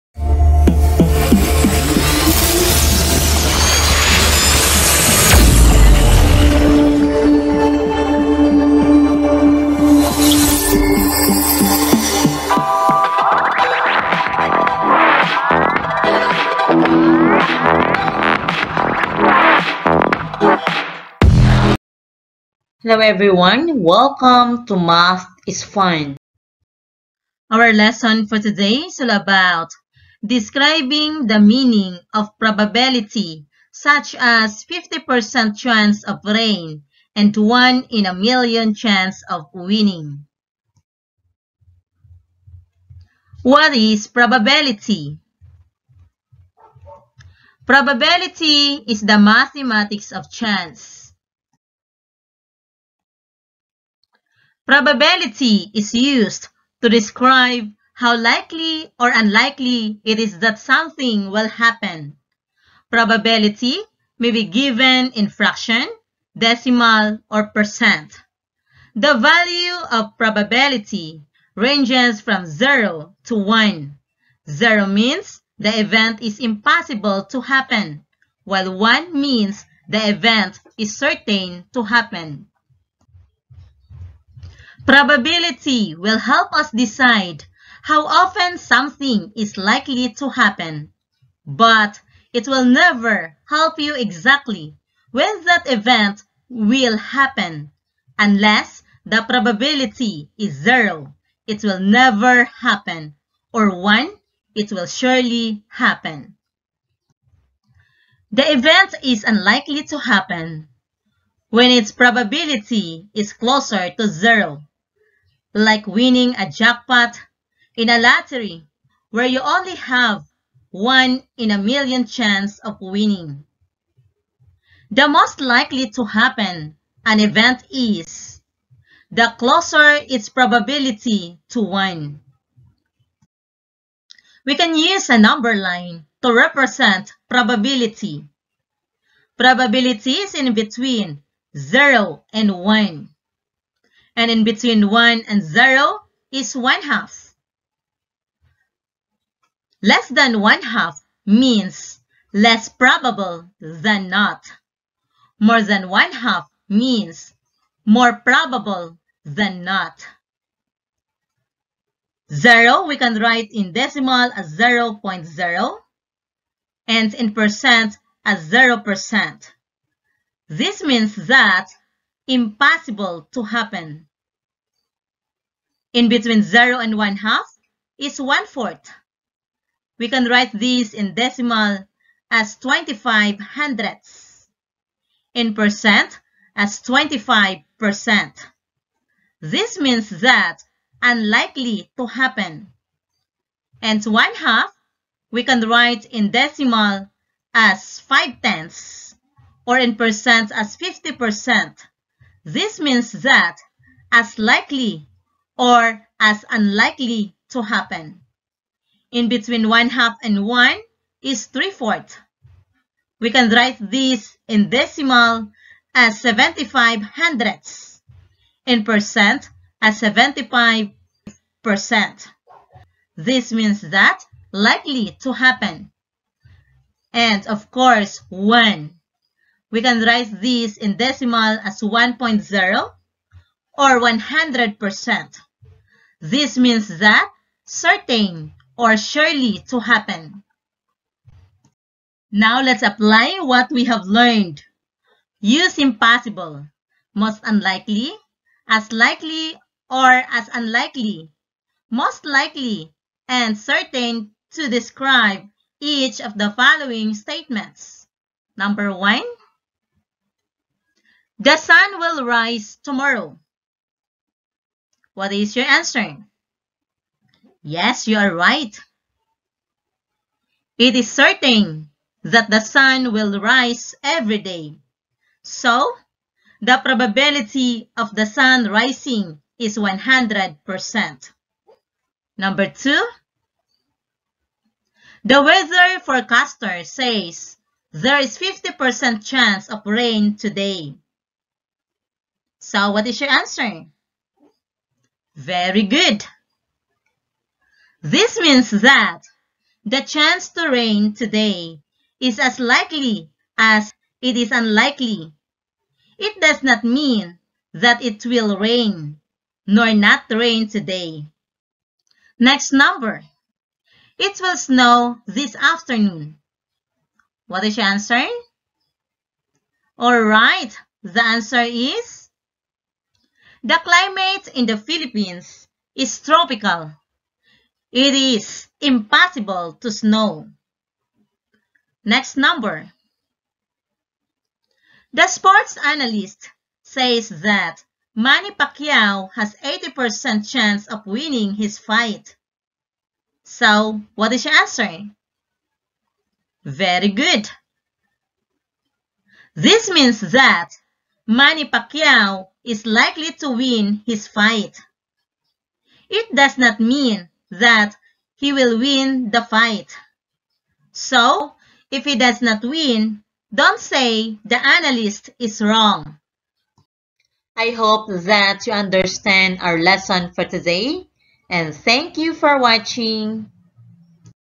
Math 6 Describing the Meaning of Probability
This video is all about describing the meaning of probability such as 50% chance of rain and a one in a million chance of winning.